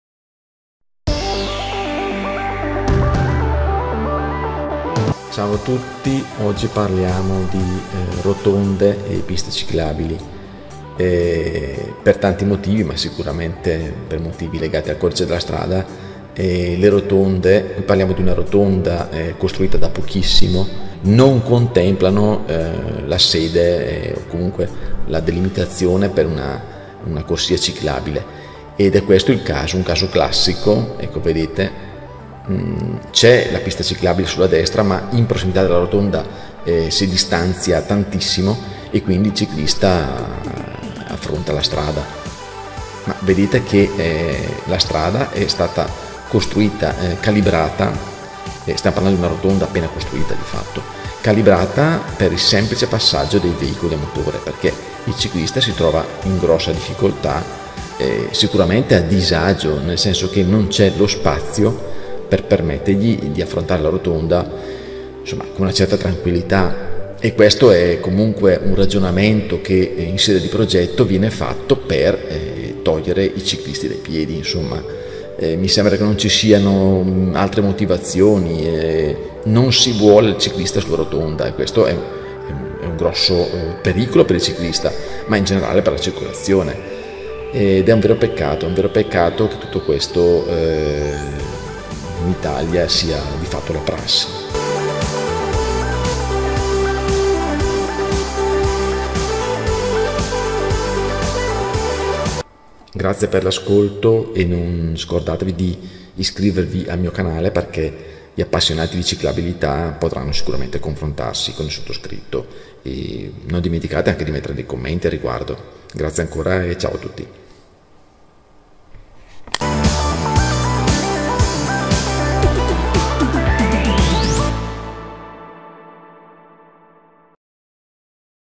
Le rotonde in Italia: nemiche delle bici!
Contro ogni logica, continua l'apartheid stradale che mette le biciclette in condizione di dover fare il doppio della distanza per andare da un punto A a un punto B.